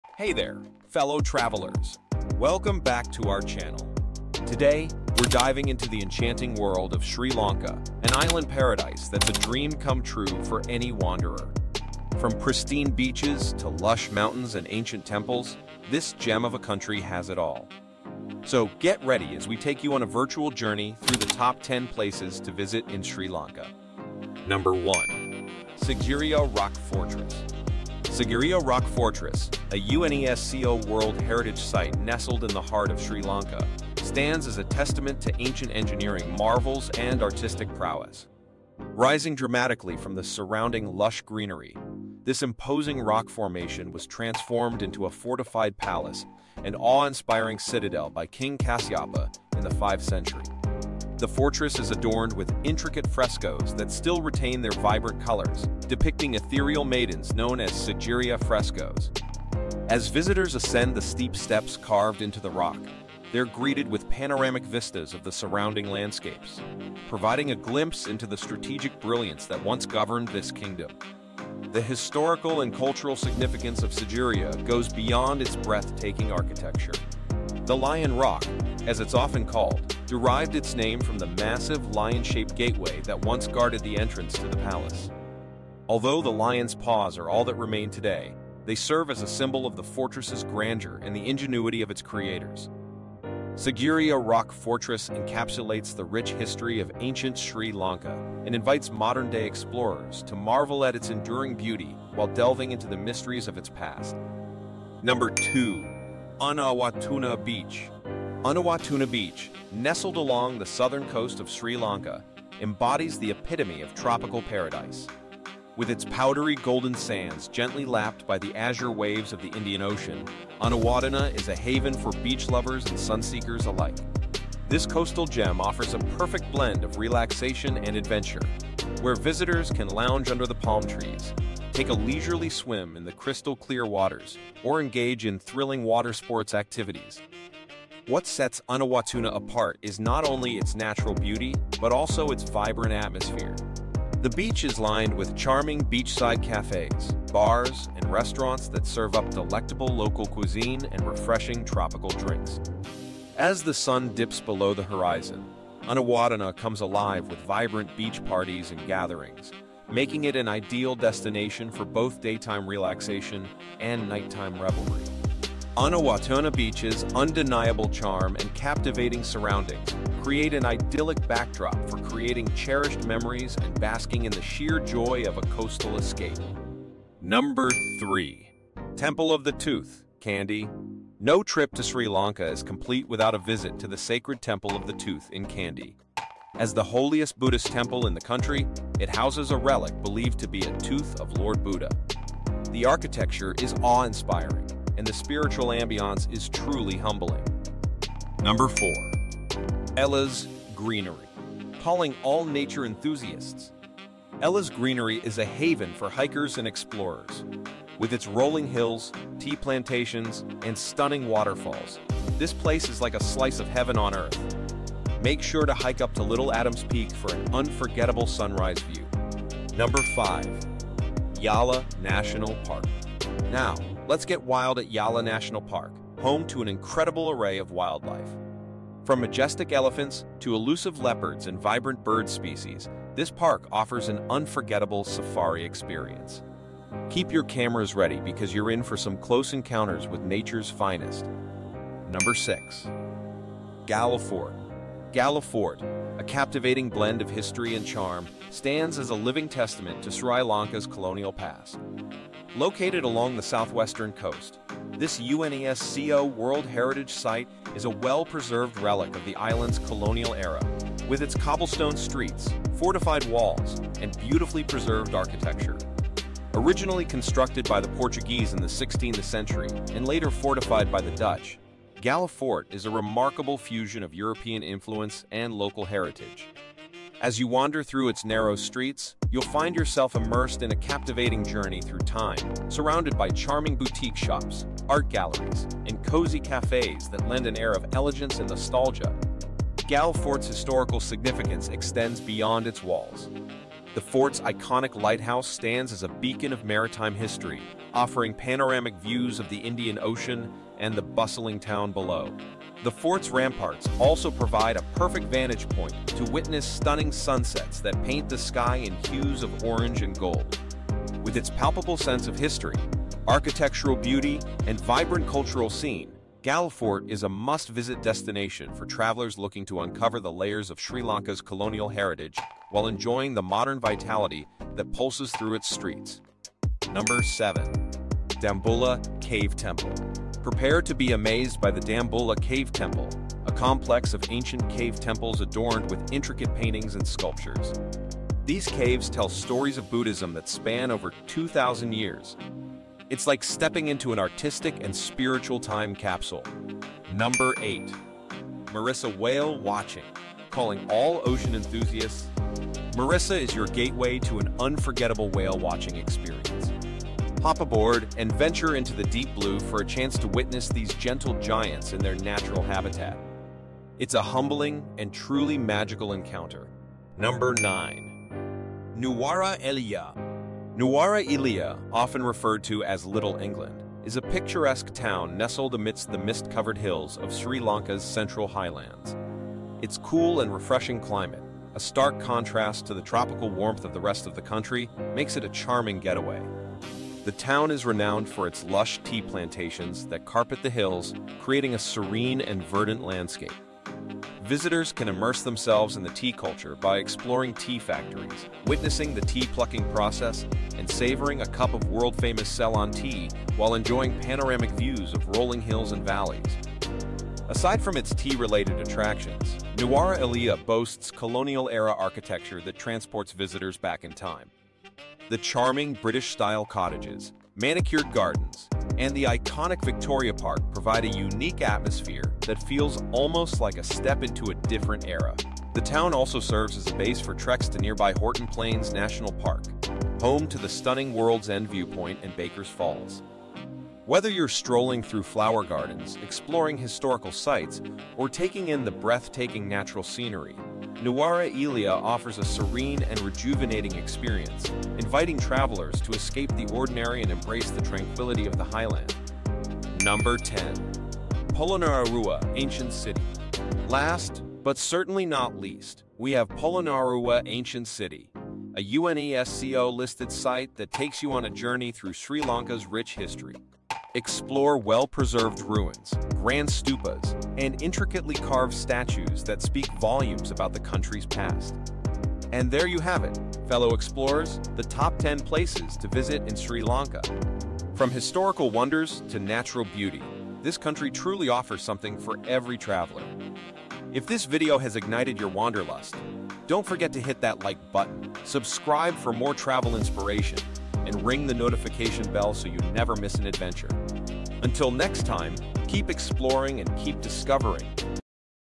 "Unveiling Sri Lanka's Hidden Gems: Must-Visit Places!"
Are you ready for an unforgettable journey through the stunning landscapes of Sri Lanka? In this video, we'll take you on a virtual tour of the top 10 must-visit places in Sri Lanka. From pristine beaches to ancient temples, lush tea plantations to vibrant markets, this comprehensive guide will inspire your next travel adventure. Join us as we explore the cultural richness and natural beauty that this island paradise has to offer!

🌍 Dreaming of a Sri Lankan getaway? 🌴 Discover the enchanting allure of Sri Lanka's top 10 destinations! From lush tea plantations to pristine beaches, this video is your ultimate travel guide. Let's embark on this journey together! ✈️